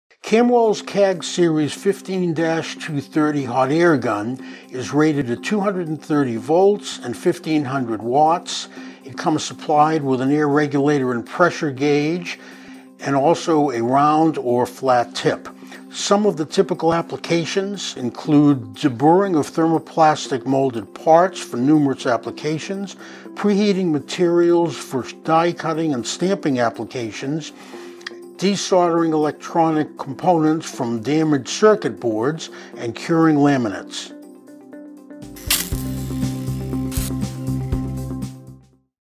Kamweld Technologies - KHAG-15-230 Hot Air Gun
Element 230v/1500w Hot Air Gun with Round or Flat Tip. The KAMWELD Hot Air Gun is supplied complete with an air regulator and pressure gauge, a 2" long, 5/8" diameter stainless steel flanged round tip with check-nut and 5 ft. of insulated cable. A stainless steel wrap-around mounting bracket permits permanent mounting to existing equipment, and the gun may be grounded through the mounting bracket.